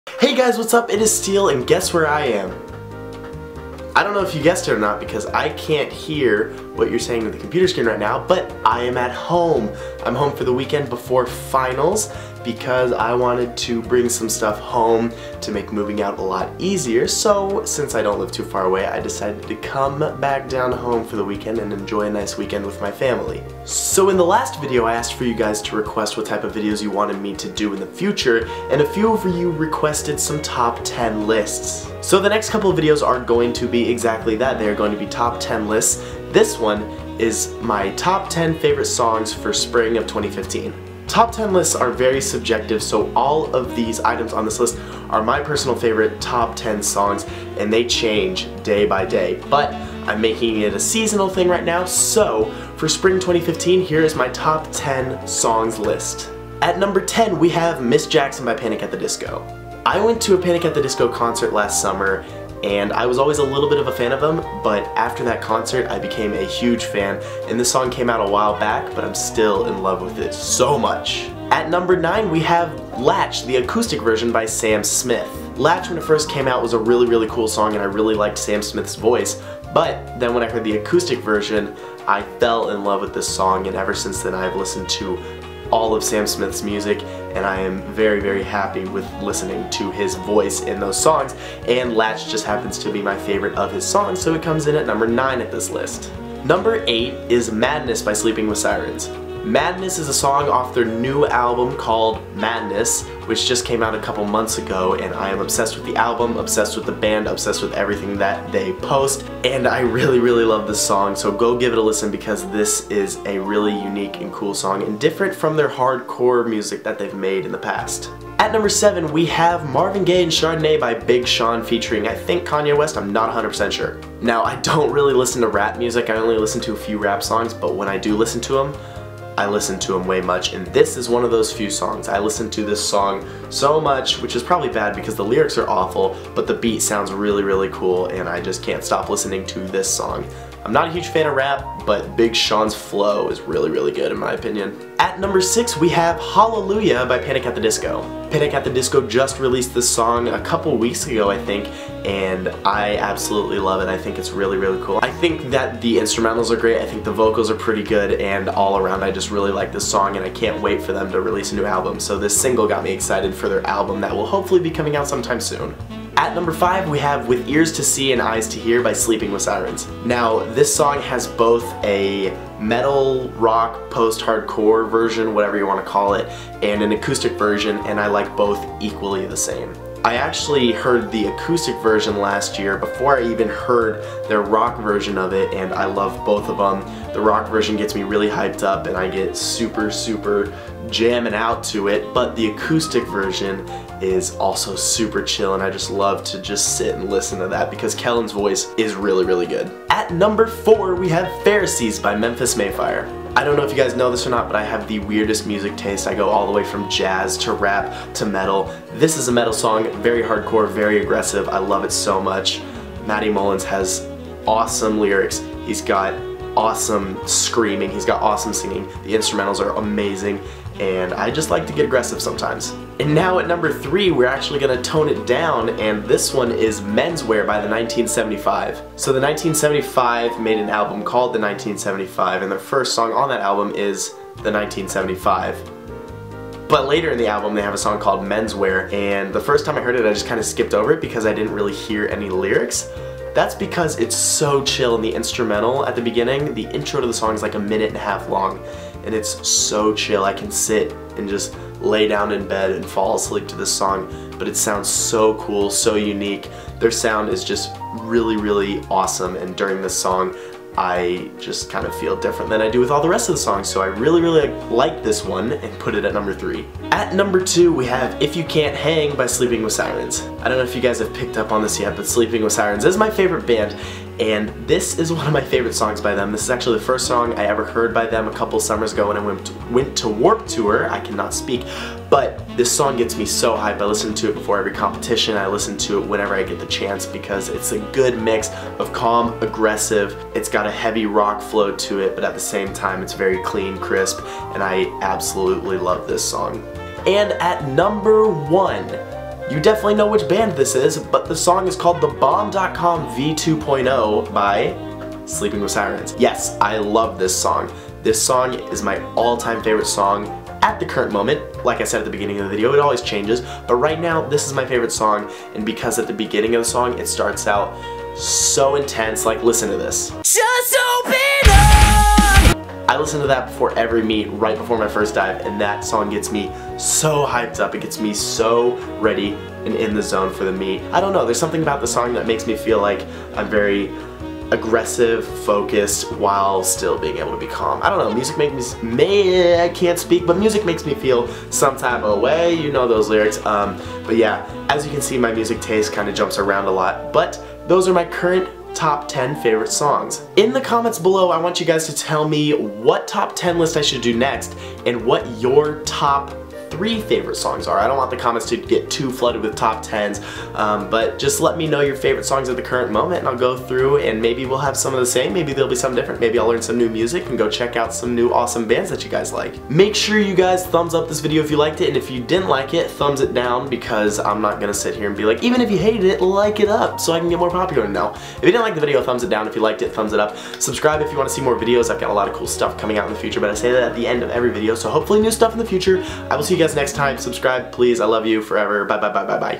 WORLD'S GREATEST SONGS!
This is a top 10 list of my favorite songs for Spring of 2015! This was a requested video by some of you so I thought "why not make it!?" I hope you like it!